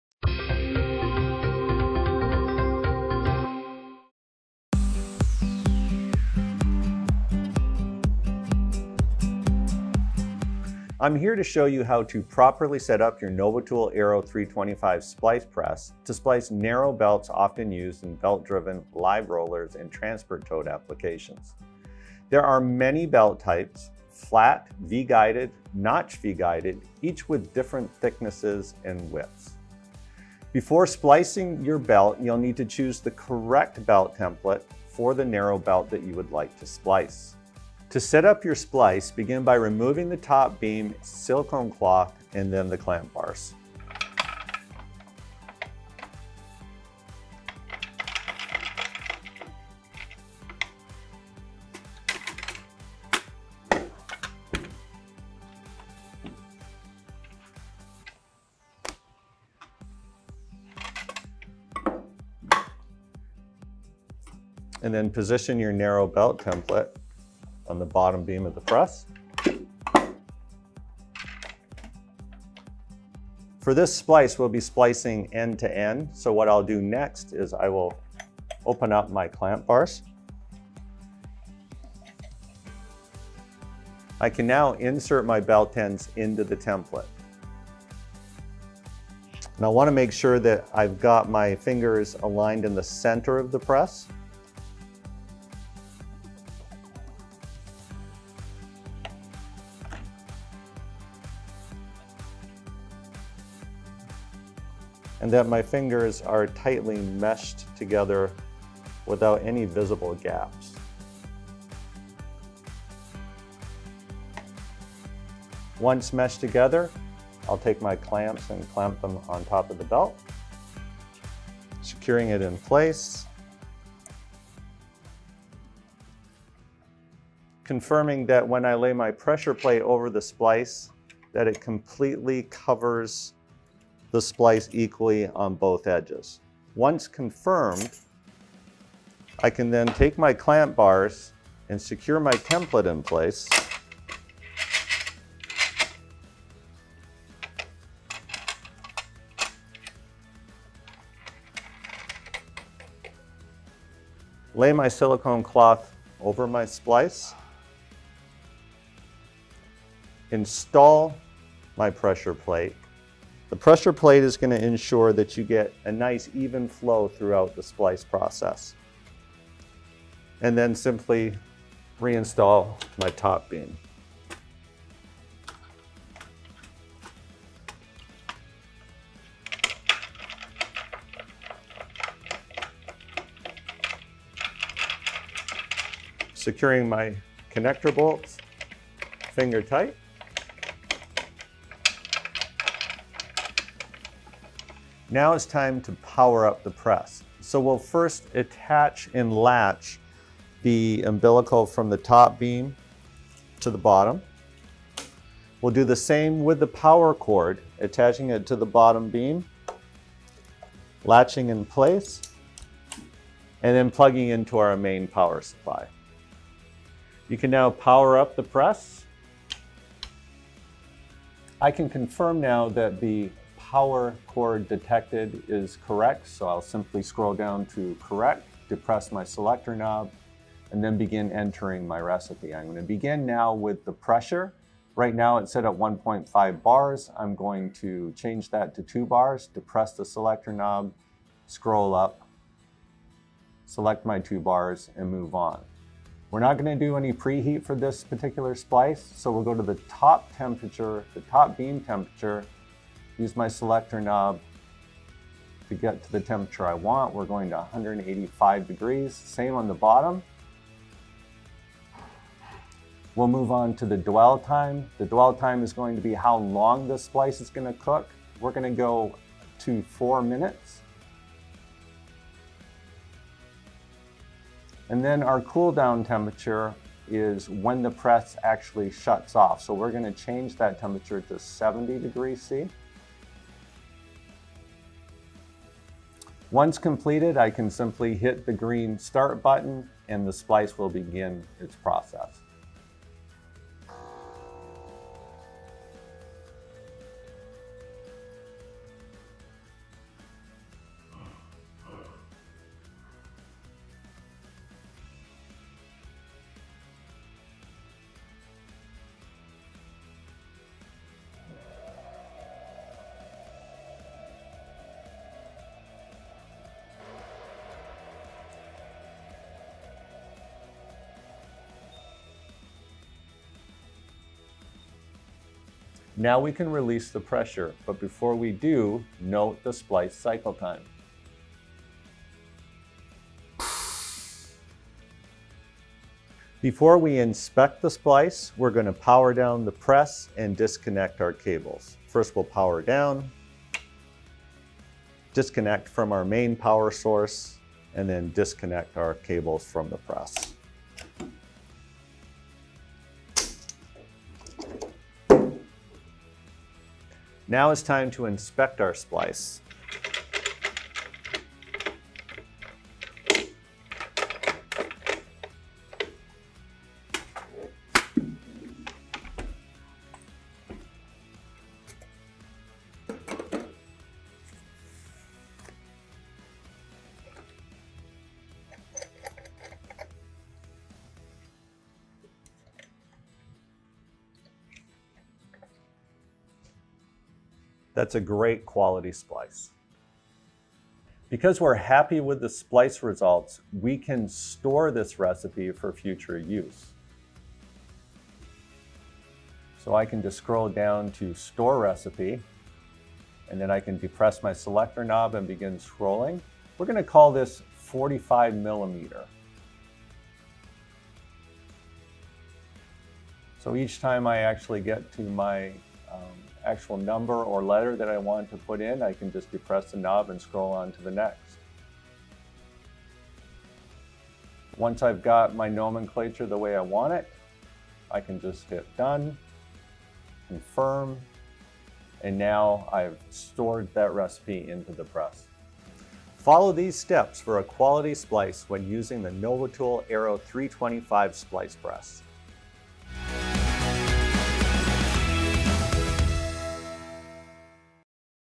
Novitool® Aero® 325 Splice Press 45mm Belt Operation
The Aero 325 Splice Press and its narrow belt templates are ideal for live roller and transport belt applications, along with other thermoplastic PVC or PU belting.
Learn how to splice a 45mm belt with Flexco Product Expert Mark Green.

V0219_enUS_0219_ Aero325SplicePressOperation _0820